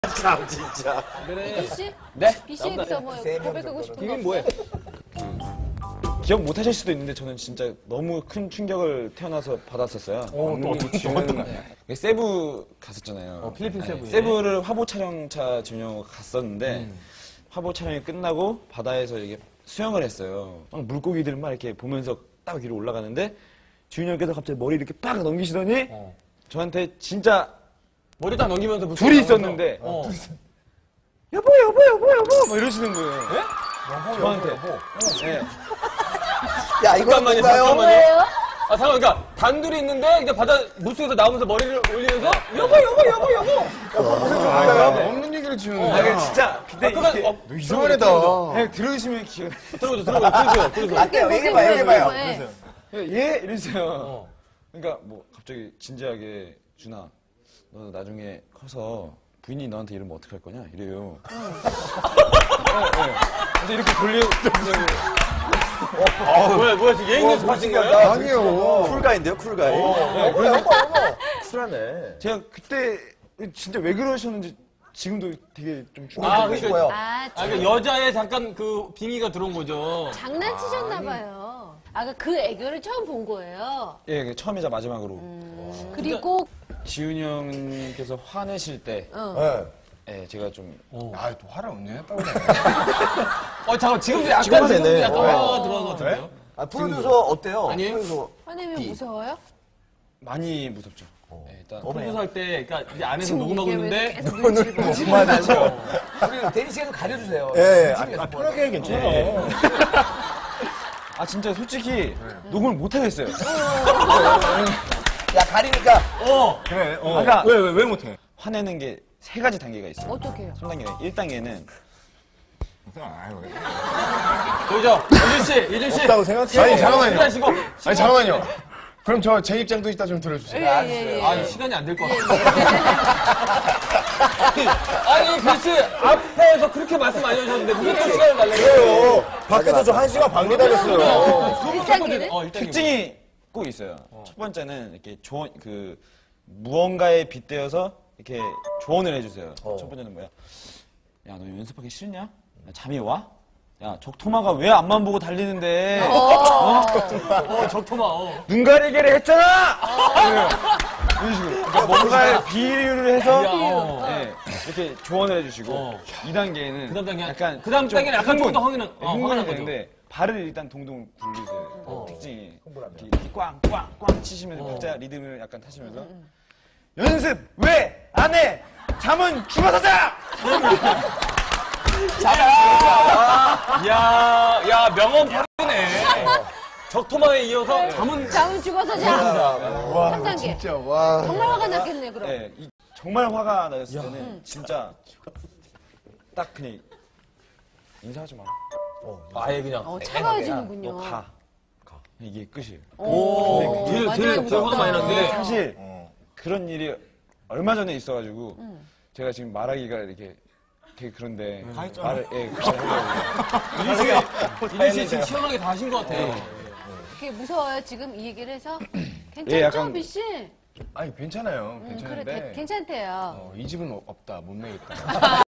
[ENG SUB] Rain Bi's 3 Stages of Anger (as told by Lee Joon)
Joon Lee of MBLAQ retells an experience with Bi that shocked him; as well as Bi's unique characteristics of anger.